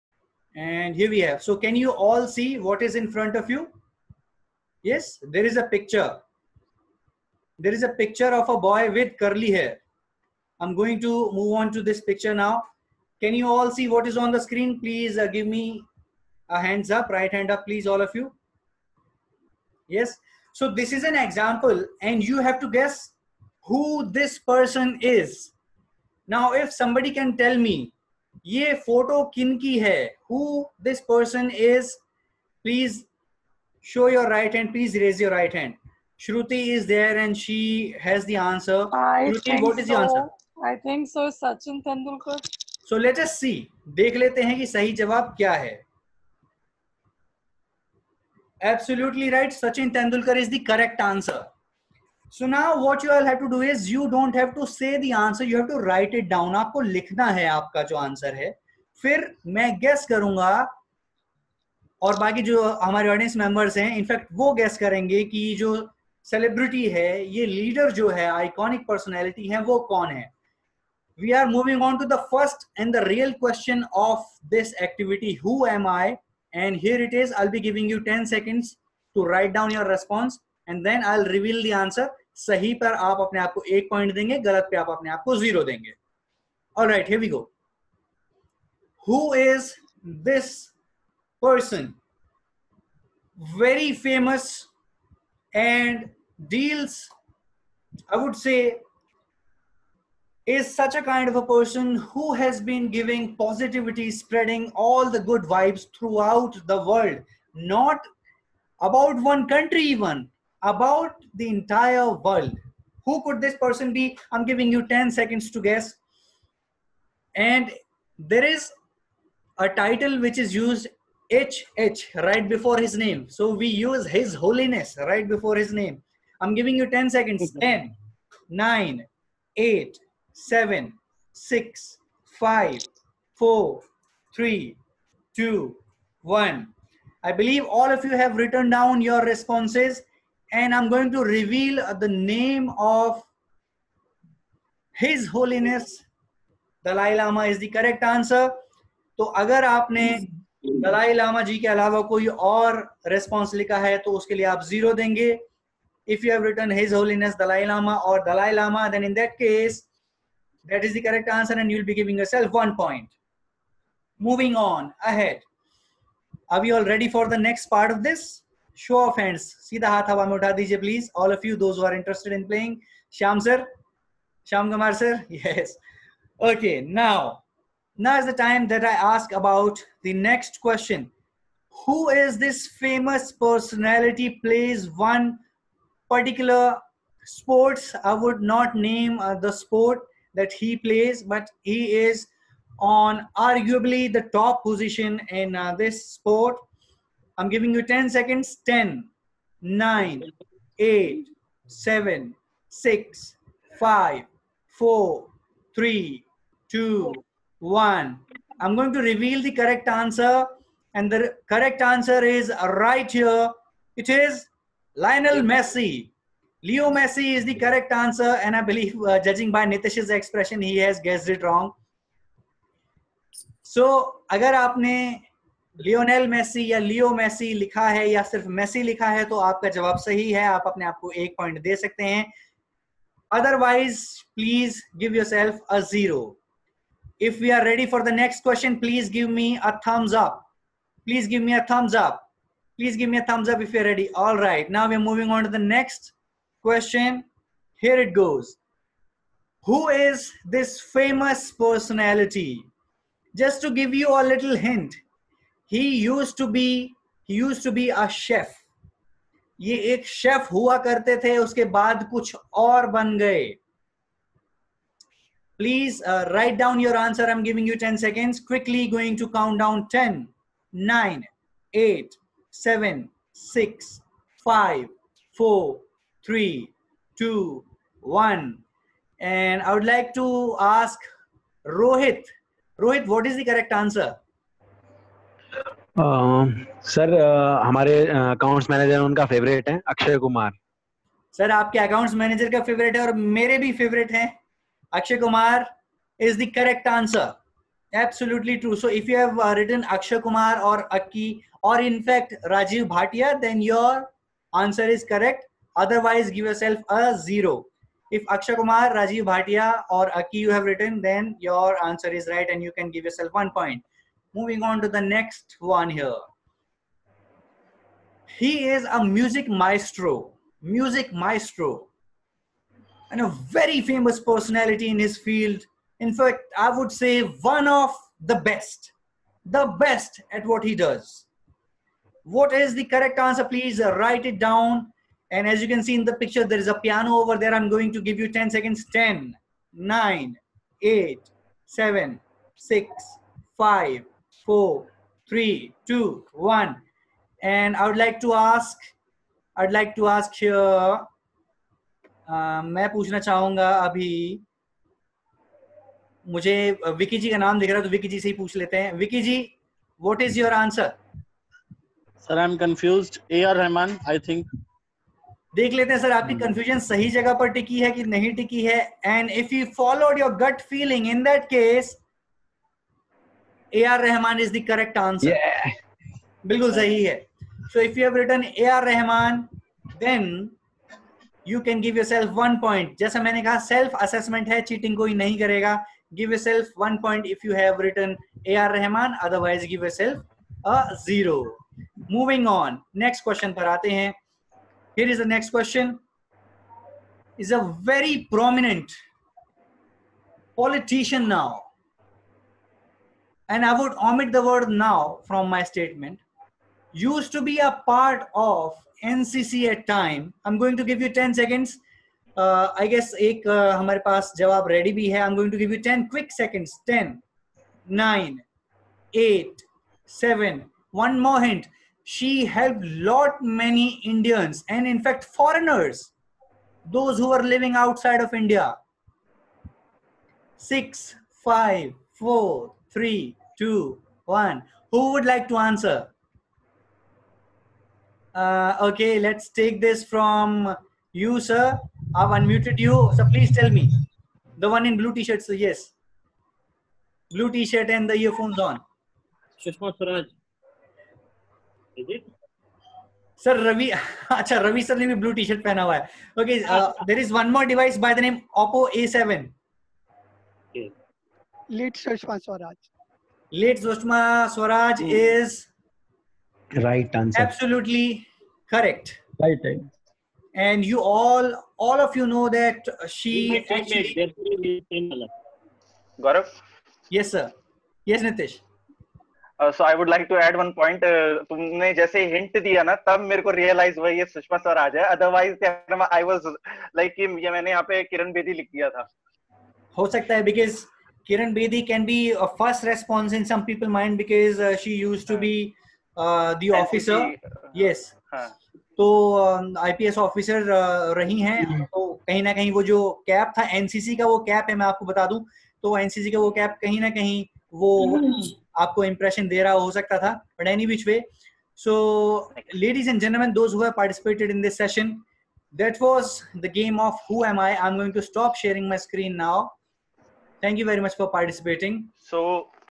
Engagement Game for Zoom Meeting by Anchor Gaurav aka Ungli Baba Who Am I?
YourDigitalAnchor Anchor Gaurav aka Ungli Baba is back with another game for your digital sessions. Share this post to help others learn & enjoy. :)

Follow him on FB LinkedIn Twitter Instagram SnapChat @EmceeGaurav